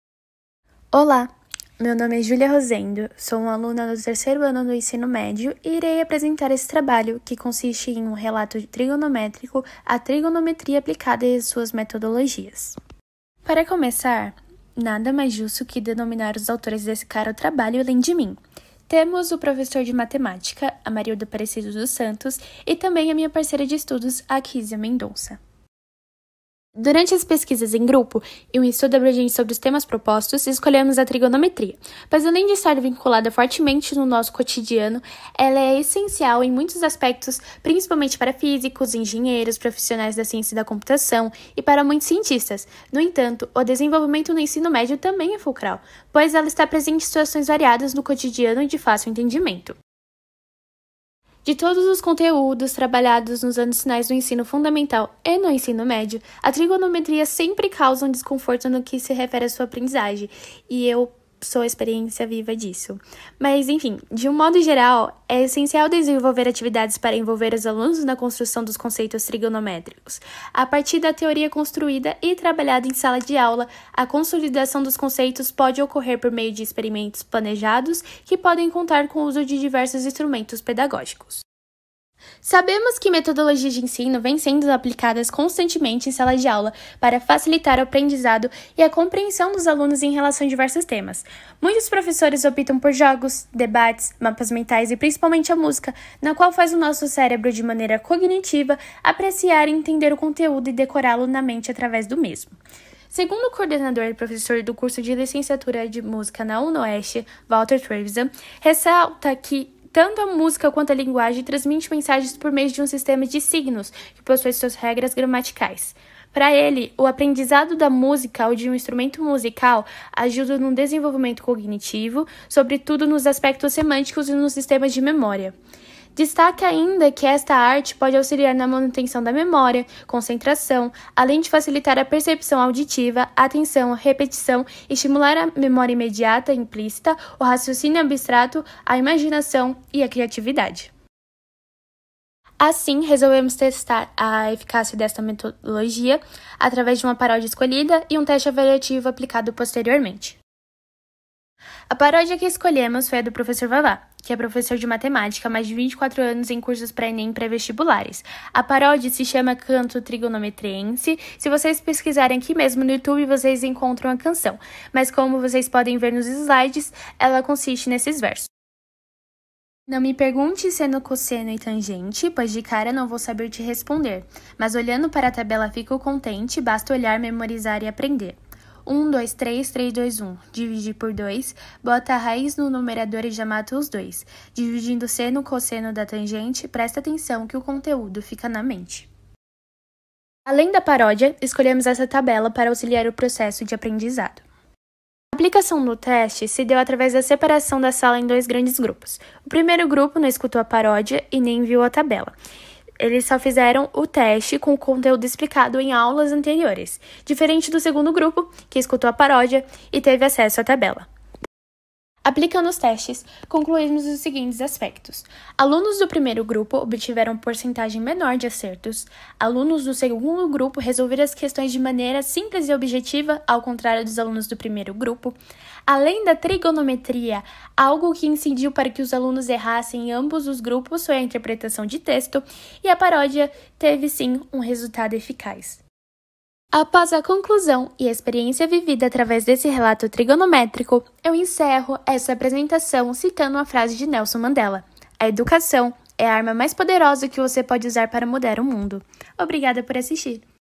Relato trigonométrico: trigonometria aplicada e as diferentes metodologias
Título do trabalho: Relato trigonométrico: trigonometria aplicada e as diferentes metodologias
Autores: Akizia Mendonça da Silva
Amarildo Aparecido dos Santos
Júlia Rosendo Gonçalves da Silva